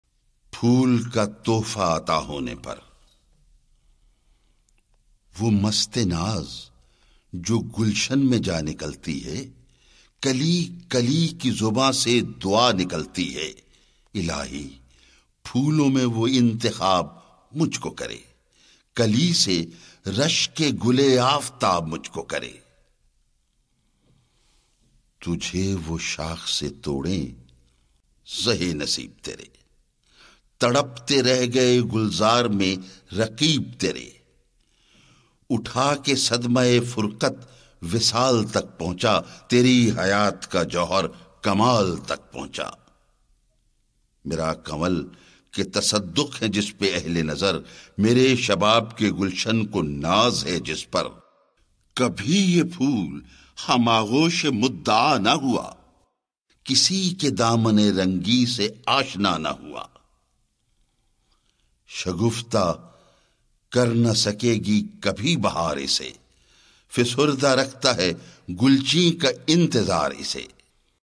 Allama Iqbal : Shujaat Reads Iqbal : Bang e Dara P-III, 08 – Phool ka tuhfa ata hone pr
پھُول کا تحفہ عطا ہونے پر
وہ مستِ ناز جو گُلشن میں جا نکلتی ہے
کلی کلی کی زباں سے دُعا نکلتی ہے
“الٰہی! پھُولوں میں وہ انتخاب مجھ کو کرے
کلی سے رشکِ گُلِ آفتاب مجھ کو کرے”
تجھے وہ شاخ سے توڑیں! زہے نصیب ترے
تڑپتے رہ گئے گُلزار میں رقیب ترے
اُٹھا کے صدمۂ فُرقت وصال تک پہنچا
تری حیات کا جوہر کمال تک پہنچا
مرا کنول کہ تصدّق ہیں جس پہ اہلِ نظر
مرے شباب کے گُلشن کو ناز ہے جس پر
کبھی یہ پھُول ہم آغوشِ مدّعا نہ ہوا
کسی کے دامنِ رنگیں سے آشنا نہ ہوا
شگفتہ کر نہ سکے گی کبھی بہار اسے
فسُردہ رکھتا ہے گُلچیں کا انتظار اسے

Renowned artist Shujaat Hashmi reads Allama Iqbal's Urdu poetical works. In this video you can listen and read the Poem “Phool ka Tuhfa ata hone pr" from Allama Iqbal's first Urdu Poetry book "Bang-e-Dara". Our Official Websites